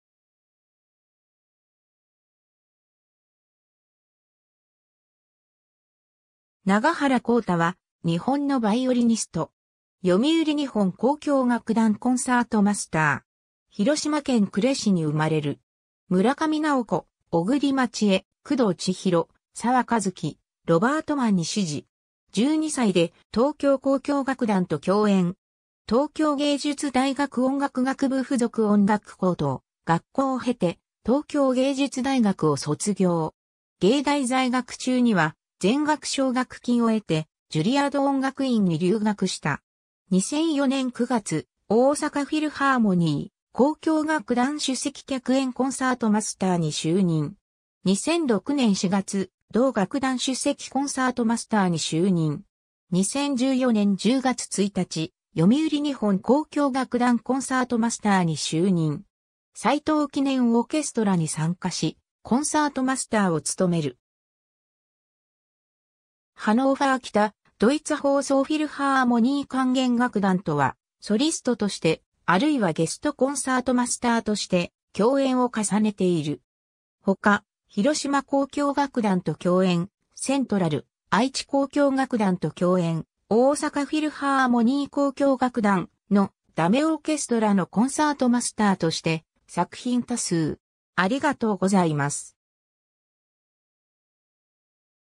長原幸太
長原 幸太（ながはら こうた、1981年 - ）は、日本のヴァイオリニスト。
読売日本交響楽団コンサートマスター（2014年10月1日 - ）。
 広島県呉市に生まれる。
村上直子、小栗まち絵、工藤千博、澤和樹、ロバート・マンに師事。
 12歳で東京交響楽団と共演。
 東京芸術大学音楽学部附属音楽高等学校を経て、東京芸術大学を卒業。
芸大在学中には、全額奨学金を得て、ジュリアード音楽院に留学した。
 2004年9月、大阪フィルハーモニー交響楽団首席客演コンサートマスターに就任。
2006年4月、同楽団首席コンサートマスターに就任（ - 2012年3月）。
 2014年10月1日、読売日本交響楽団コンサートマスターに就任（現任）。
 サイトウ・キネン・オーケストラに参加し、コンサートマスターを務める。
ハノーファー北ドイツ放送フィルハーモニー管弦楽団とは、ソリストとして、あるいはゲスト・コンサートマスターとして共演を重ねている。
 他、広島交響楽団と共演（じゃけん！モーツァルト 2006/10/4）、セントラル愛知交響楽団と共演（日本の管弦楽曲100周年ライヴ！ 2013/12/19）、大阪フィルハーモニー交響楽団、のだめオーケストラのコンサートマスターとして作品多数